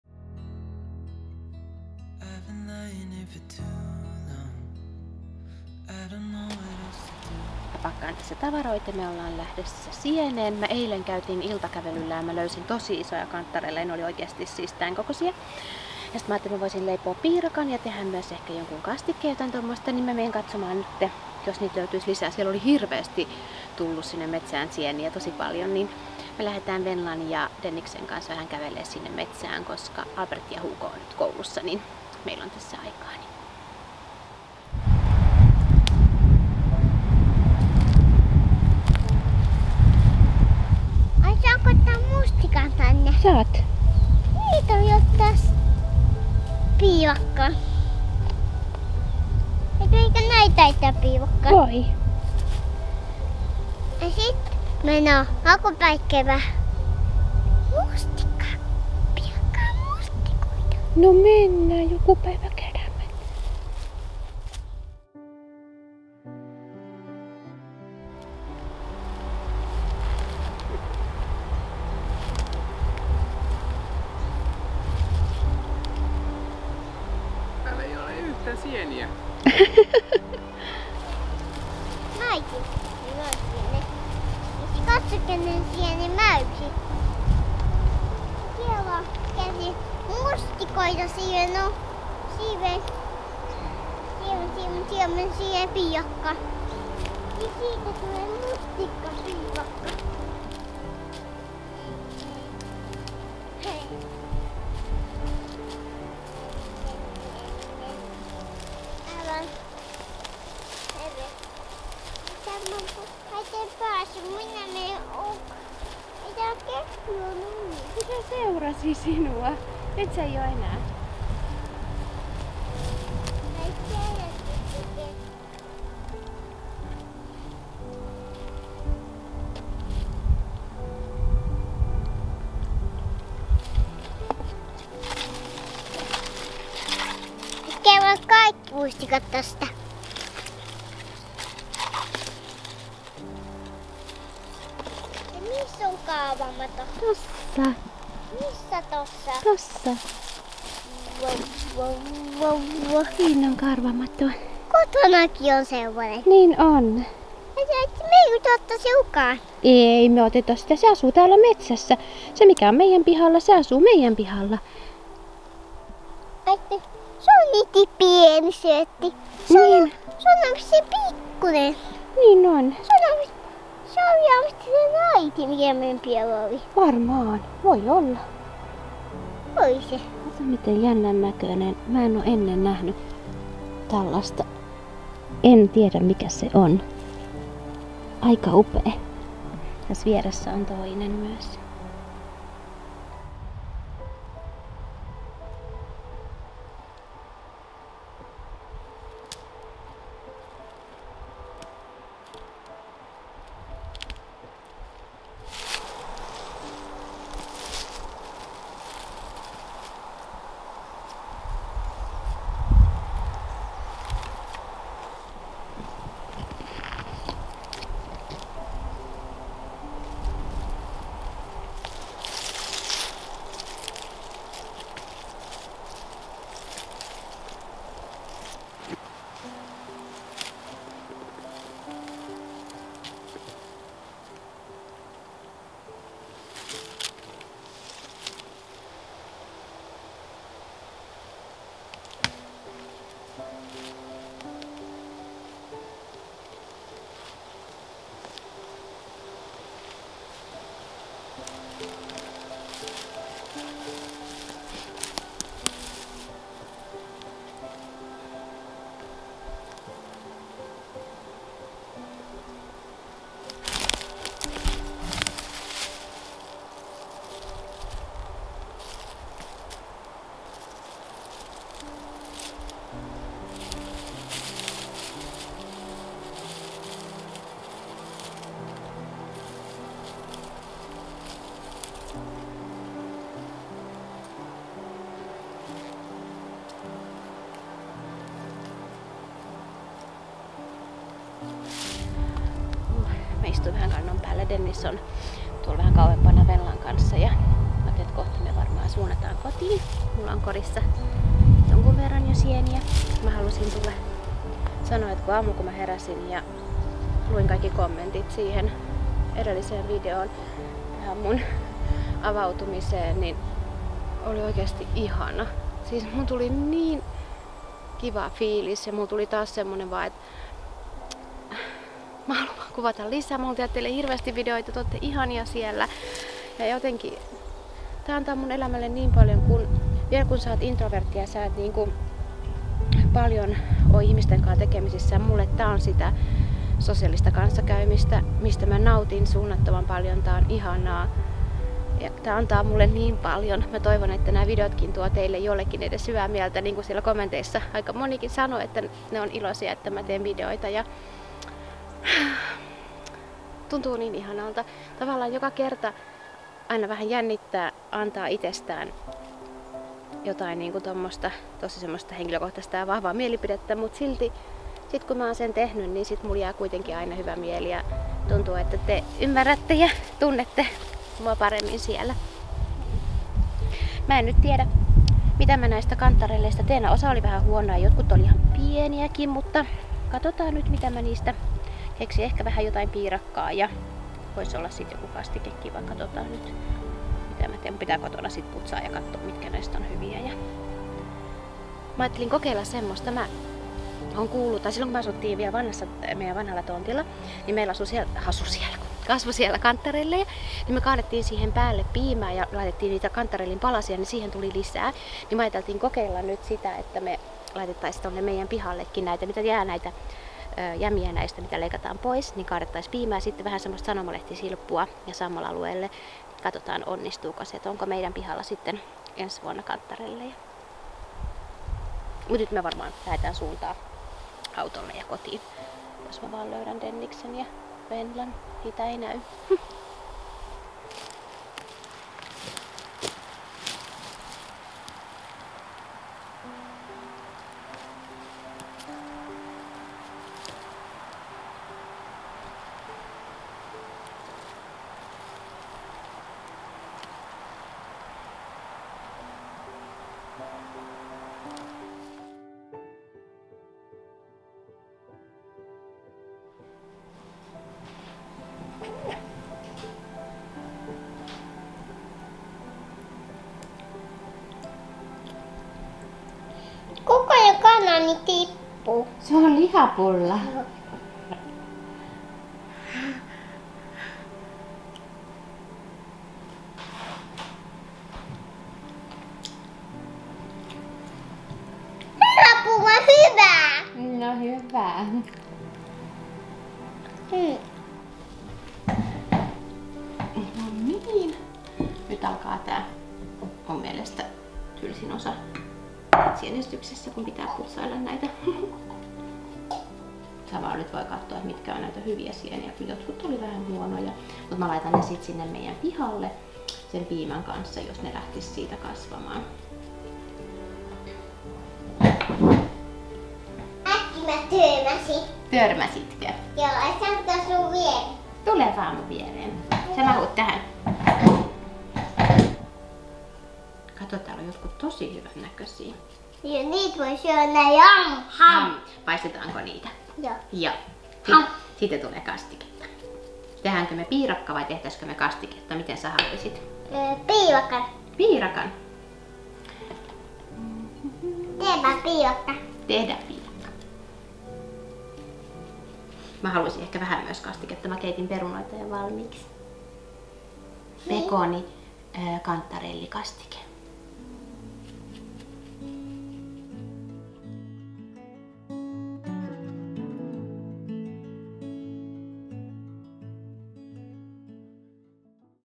MY DAY // Vlogi sienestys elämää maalla
Kävimme ihanassa sienimetsässä ja löysimme hyviä sieniä.